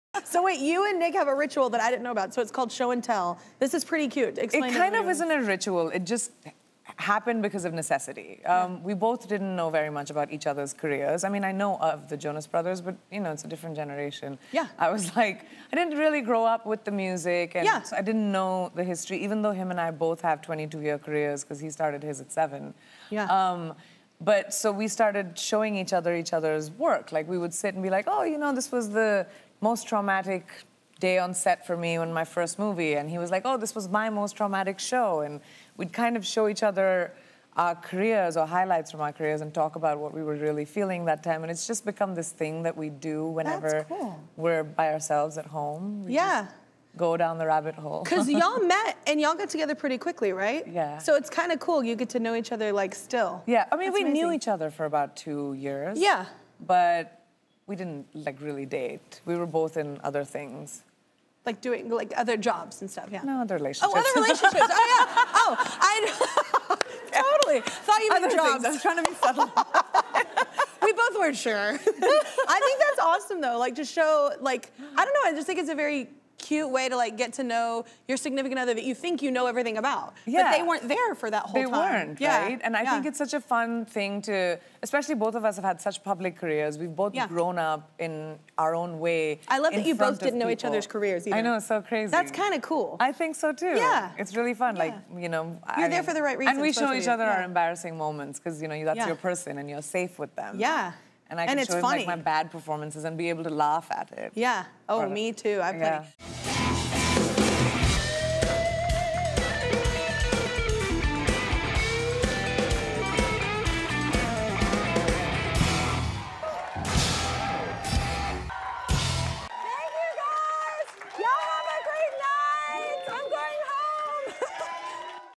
Priyanka Chopra Jonas & Nick Jonas' Adorable 'Show & Tell' Ritual
Priyanka Chopra Jonas opens up about her adorable "show and tell" ritual with her husband Nick Jonas, where they share highlights and lowlights of each others' careers that they weren't aware of before getting together. Tune in today for more fun with Priyanka Chopra Jonas.